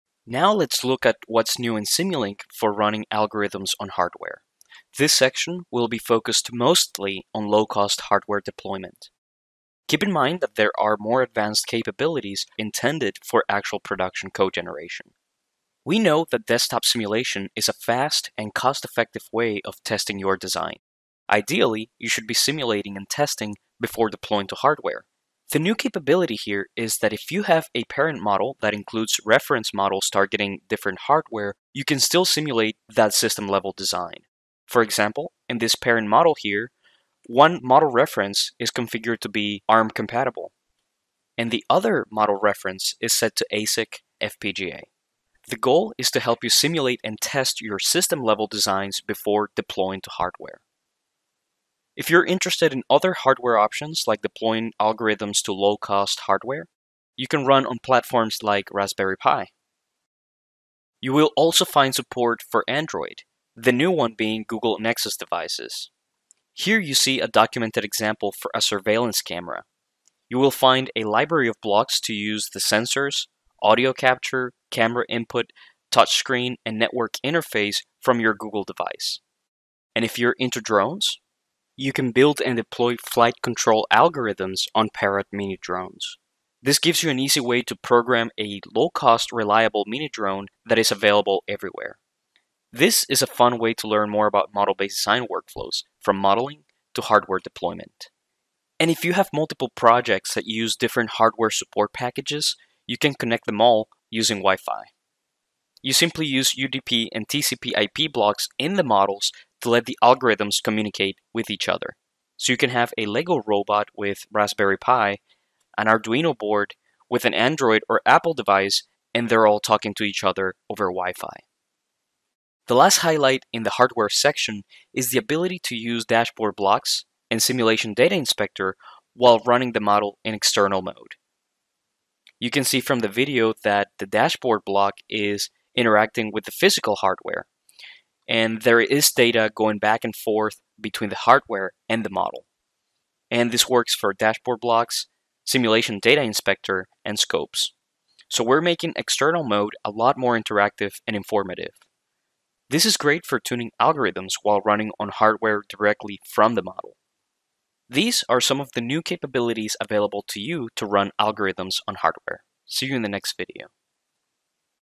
New Ways to Work in Simulink, Part 7: Run Algorithms on Hardware
You can run algorithms directly on a variety of hardware options, including Arduino® boards, Raspberry Pi™ computers, LEGO® MINDSTORMS® EV3, and Android™ and iOS smartphones and tablets. This video walks through the latest features that enable you to deploy your algorithms.

Additional Resources:

- Build Something! MATLAB and Simulink for Hardware Projects